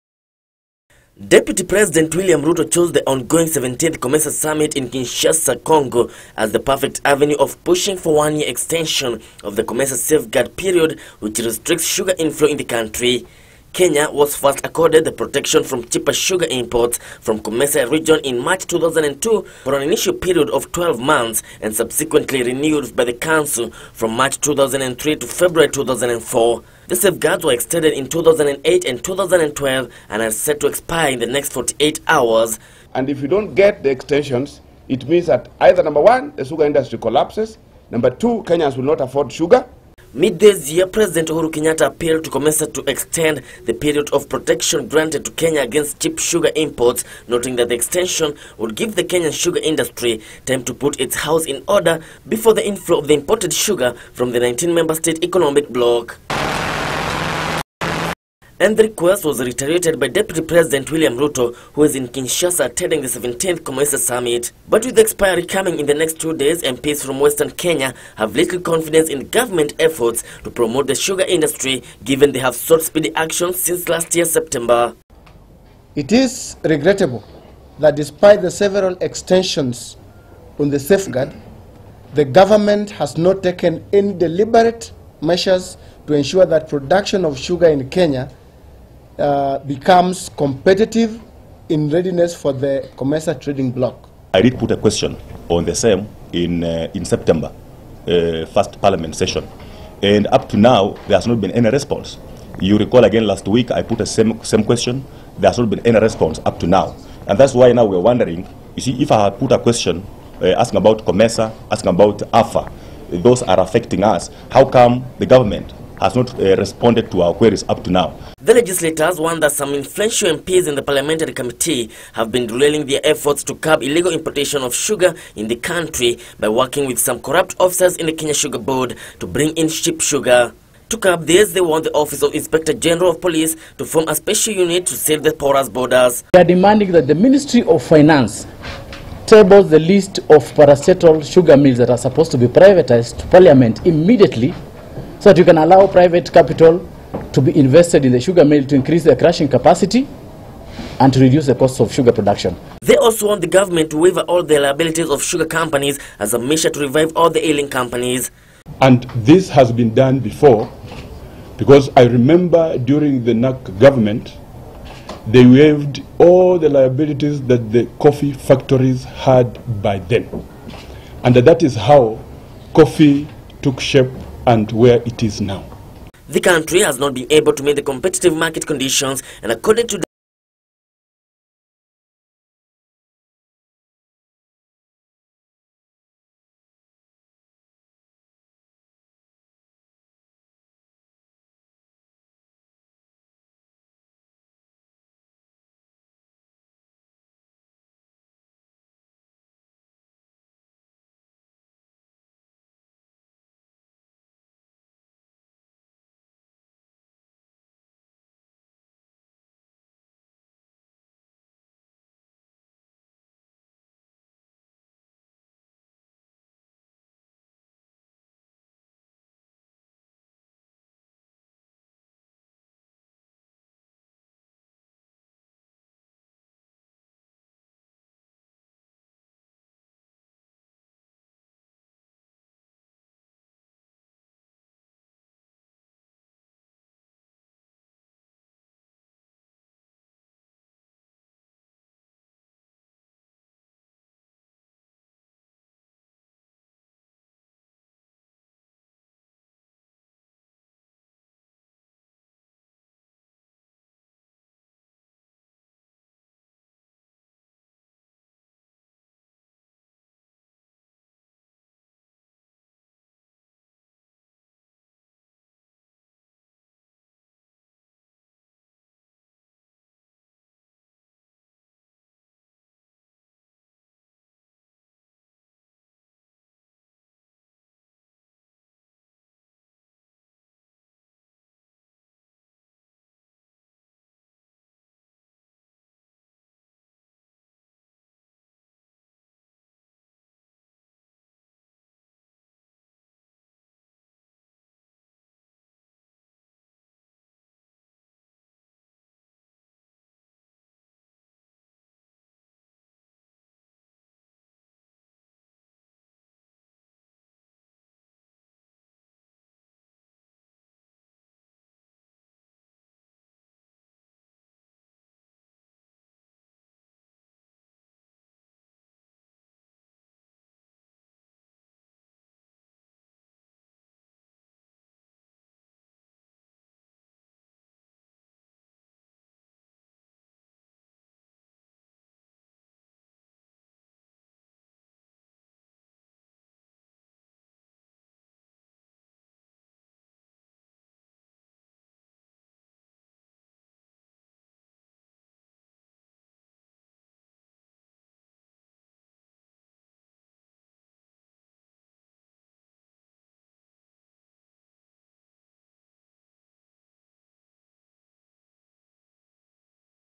Kenya appeals to COMESA for sugar safeguards
Deputy President William Ruto is now pushing for a one year extension of the COMESA safeguard period which restricts sugar inflow in the country. Ruto's push comes at a time Western Kenya legislators have united in questioning the government's commitment in protecting and promoting sugarcane industry, as well as waiver liabilities of all sugar companies as a measure to revive the sector. Report by Kevin Wachira.